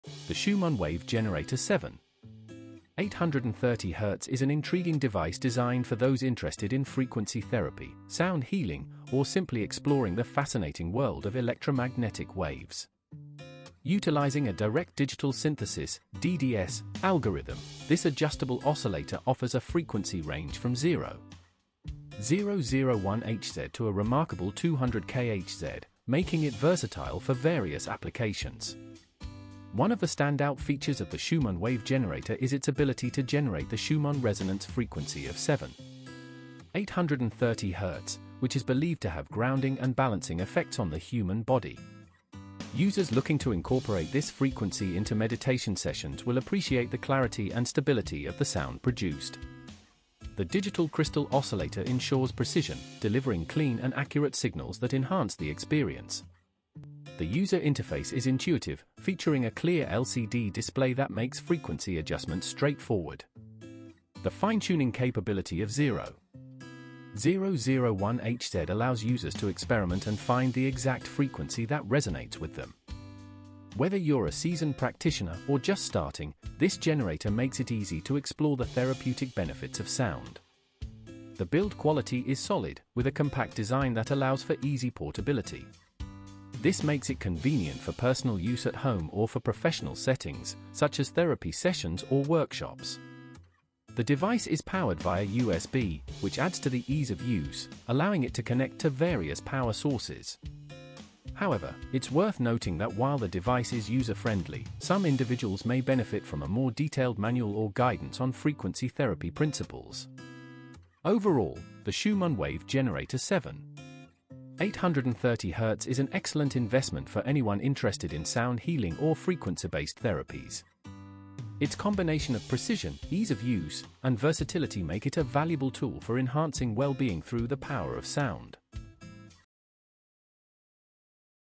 Schumann Wave Generator 7.830Hz DDS Algorithm Adjustable 0.001Hz-200KHz Digital Crystal Review
The Schumann Wave Generator 7.830Hz is an intriguing device designed for those interested in frequency therapy, sound healing, or simply exploring the fascinating world of electromagnetic waves. Utilizing a Direct Digital Synthesis (DDS) algorithm, this adjustable oscillator offers a frequency range from 0.001Hz to a remarkable 200KHz, making it versatile for various applications.

One of the standout features of the Schumann Wave Generator is its ability to generate the Schumann resonance frequency of 7.830Hz, which is believed to have grounding and balancing effects on the human body. Users looking to incorporate this frequency into meditation sessions will appreciate the clarity and stability of the sound produced. The digital crystal oscillator ensures precision, delivering clean and accurate signals that enhance the experience.

The user interface is intuitive, featuring a clear LCD display that makes frequency adjustments straightforward. The fine-tuning capability of 0.001Hz allows users to experiment and find the exact frequency that resonates with them. Whether you’re a seasoned practitioner or just starting, this generator makes it easy to explore the therapeutic benefits of sound.

The build quality is solid, with a compact design that allows for easy portability. This makes it convenient for personal use at home or for professional settings, such as therapy sessions or workshops. The device is powered via USB, which adds to the ease of use, allowing it to connect to various power sources.

However, it’s worth noting that while the device is user-friendly, some individuals may benefit from a more detailed manual or guidance on frequency therapy principles. Overall, the Schumann Wave Generator 7.830Hz is an excellent investment for anyone interested in sound healing or frequency-based therapies. Its combination of precision, ease of use, and versatility make it a valuable tool for enhancing well-being through the power of sound.